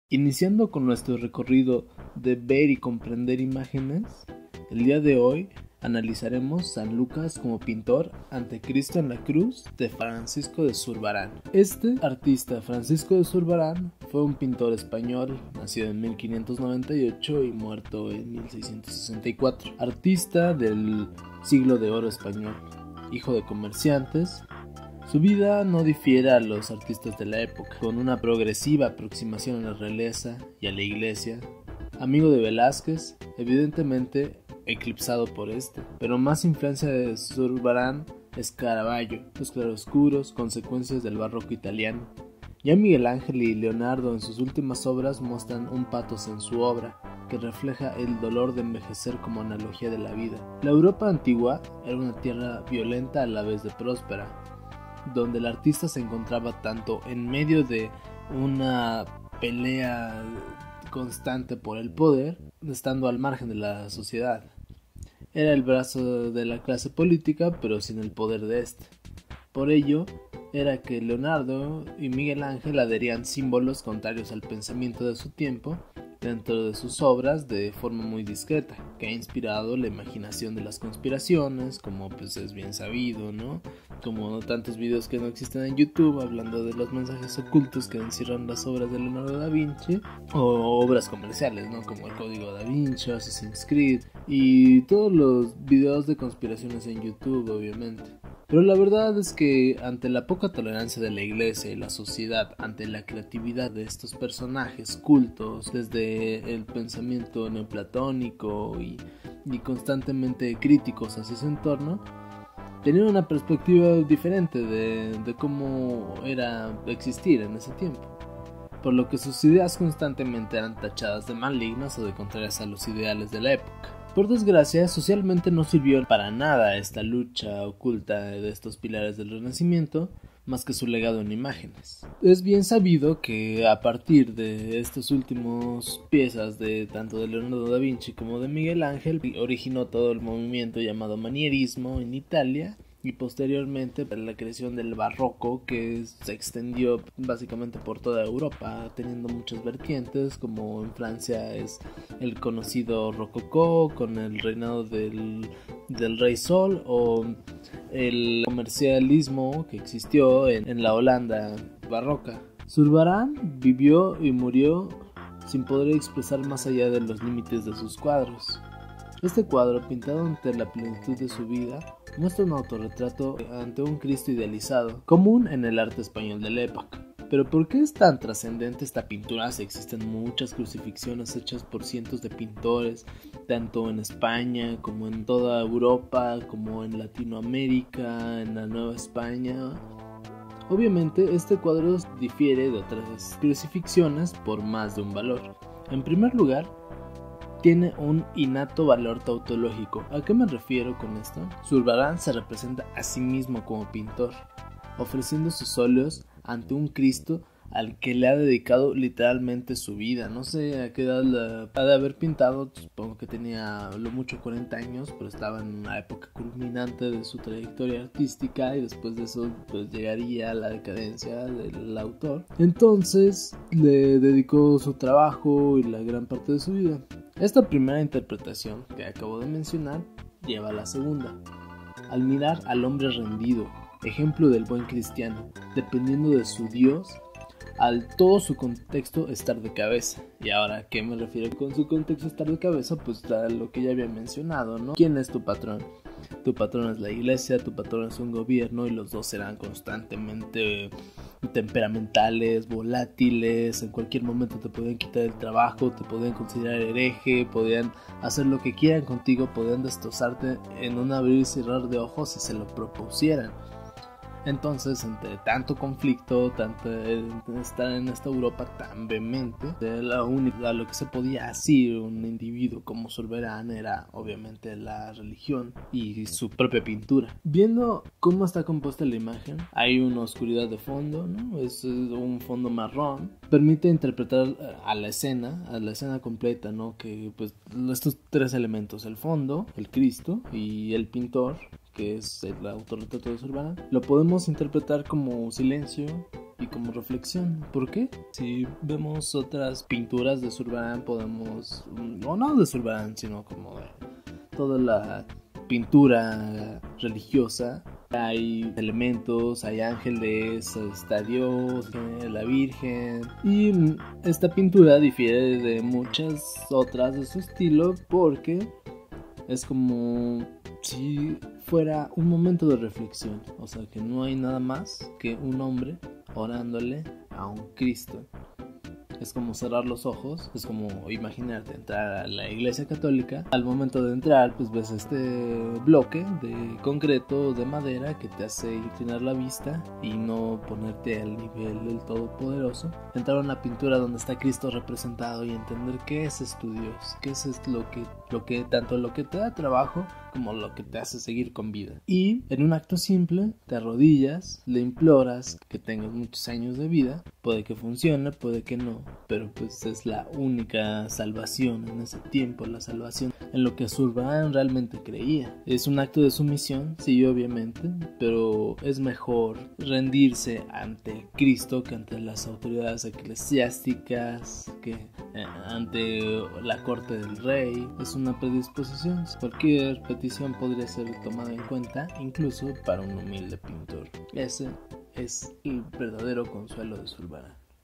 Francisco de Zurbarán.
¡Hola mundo! Esta serie consistirá en reflexiones personales respecto a "obras de arte" de todos los movimientos artisticos, desde la prehistoria al arte actual.

En esta ocasión le atañe a... Francisco de Zurbarán.